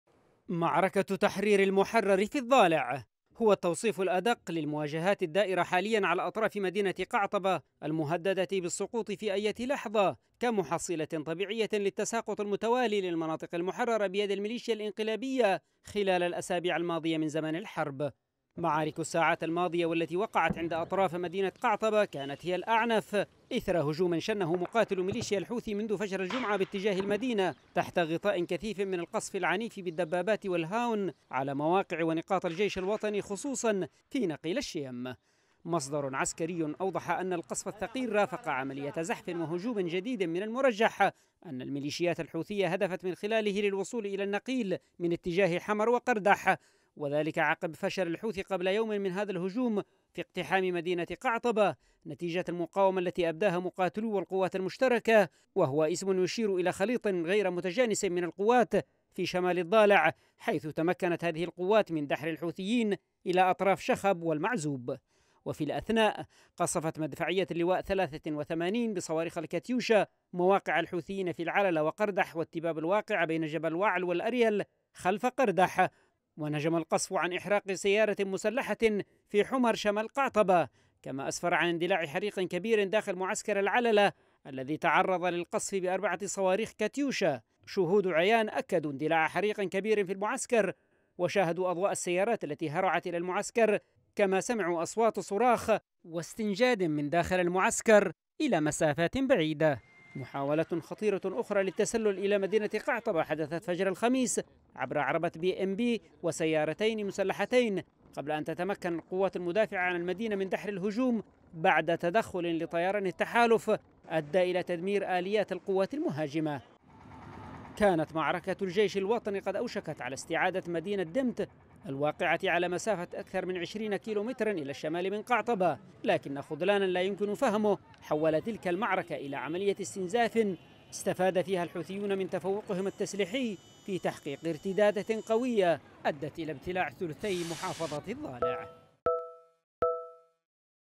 معارك عنيفة تدور على مشارف مدينة قعطبة بالضالع والجيش فشل هجومين للميلشيا | تقرير: ياسين التميمي
أعلن الجيش الوطني مقتلَ عددٍ من عناصر مليشيا الحوثي بينهم قيادي ميداني شمالي محافظة الضالع.
وذكر الجيش في موقعه الرسمي سبتمبر نت، أن قواته أحبطت هجوماً لمليشيا الحوثي في نقيل الشيم بجبهة مريس وقتلت عدداً منهم...